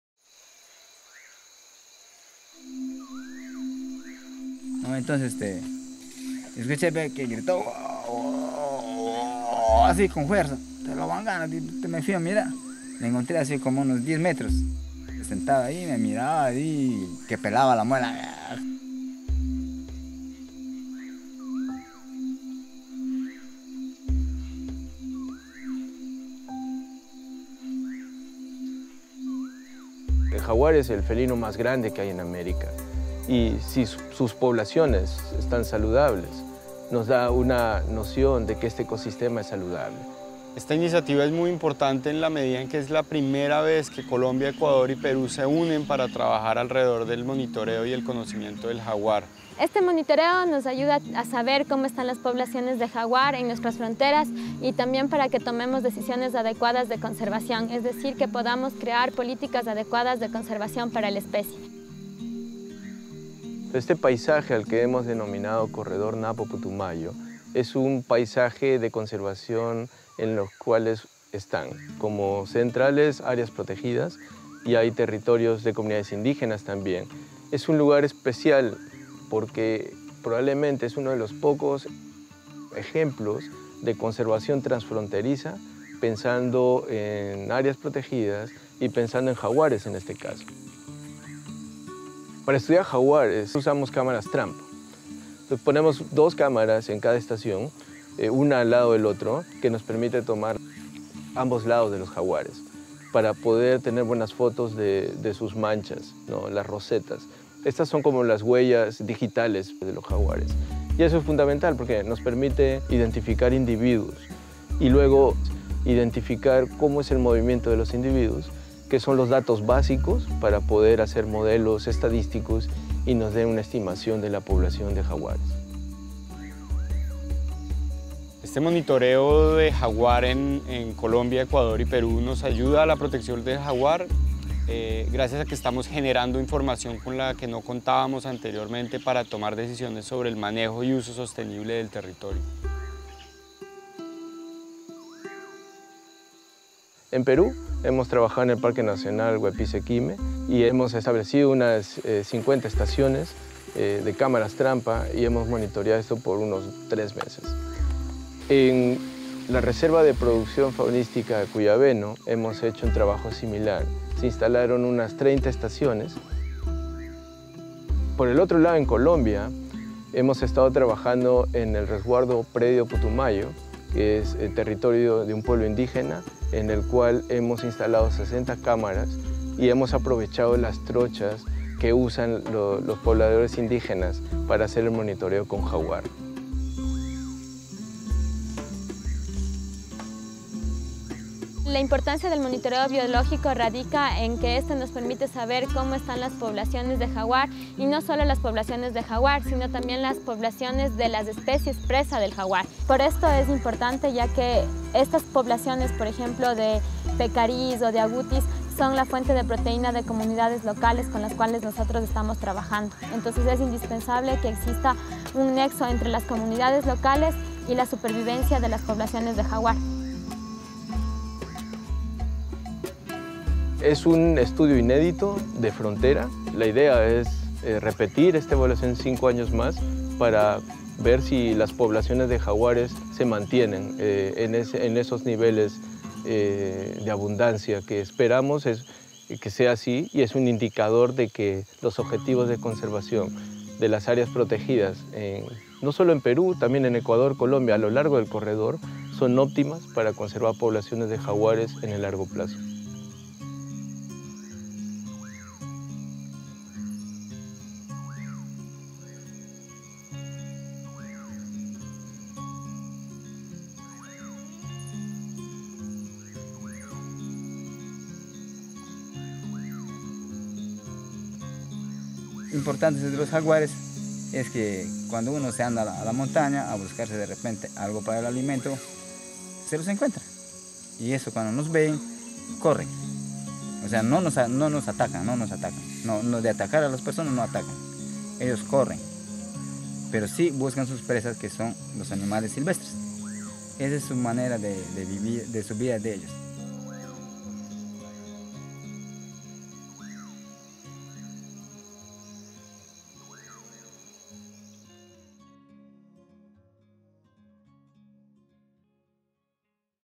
The jaguar in the Northern Amazon Corridor
Know more about our tri-national monitoring of the jaguar and its preys in the Northern Amazon Corridor among Colombia, Ecuador y Perú. This process is a big effort to protect the amazing feline and forests in the region.